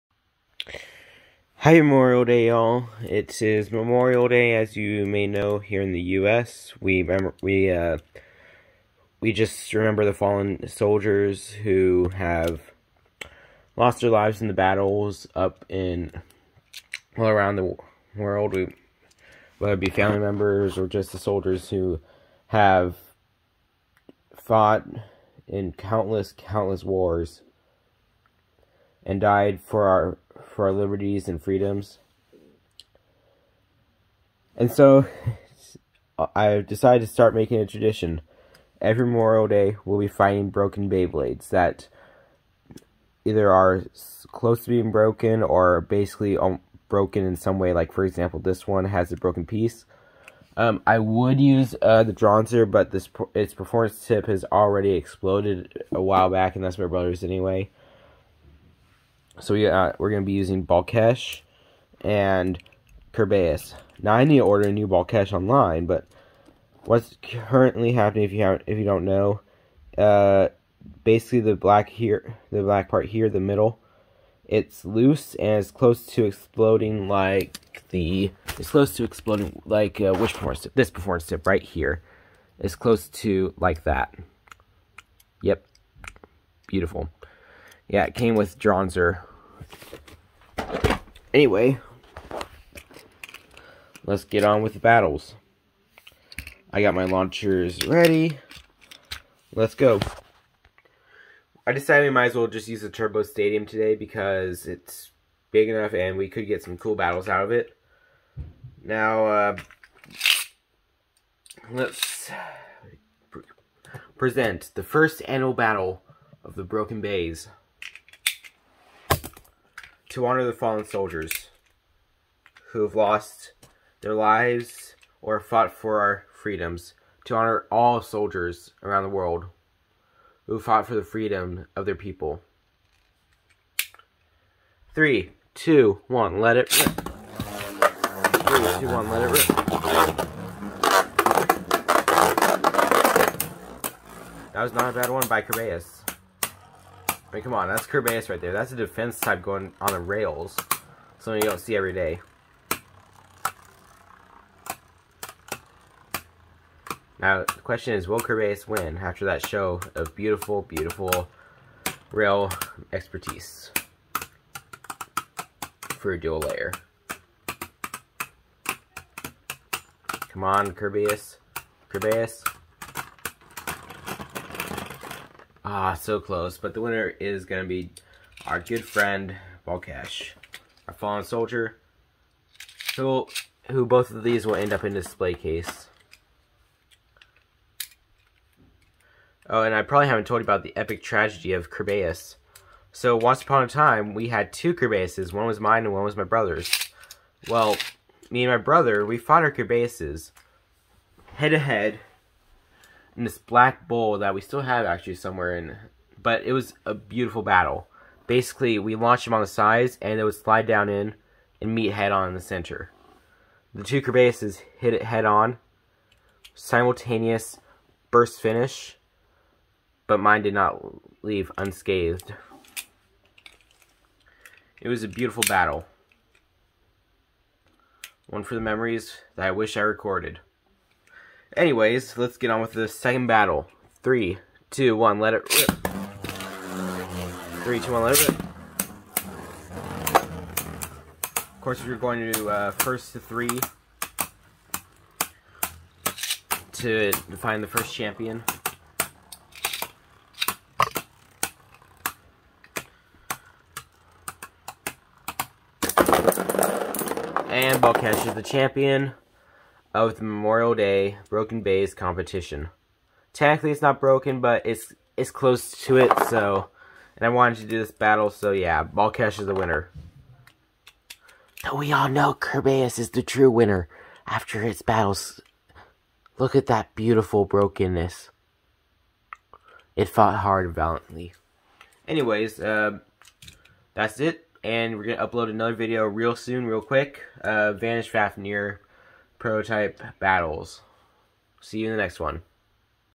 1st Annual Memorial Day Broken Bey Challenge
This Memorial Day I decided to honor the soldiers who fought valiantly for the freedoms of all people, we would make a broken bey competition, where usable beys that have broken in some way would compete for glory in 3 point battles.